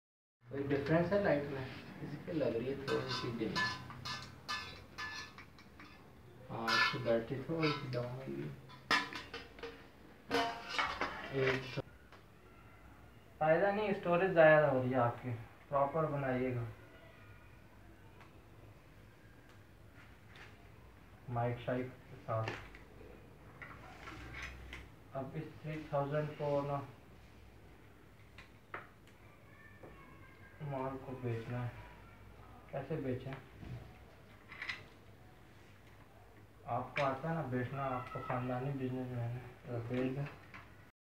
Video photography accessories videomaking in low budget | wholesale
Mobiles photography accessories sale/videomaking inlowbudget | wholesaler
Mobiles photographers need necessary accessories to make an effective video. Arrangements of camera and lights for photography, different tripods and lights are essential components . LED lights like ring lights in different sizes use for indoor video making. Micro phones in different qualities are also use for quality recording and voice over popurse.|ZahoorTariq |KhurramAliSiddiqui|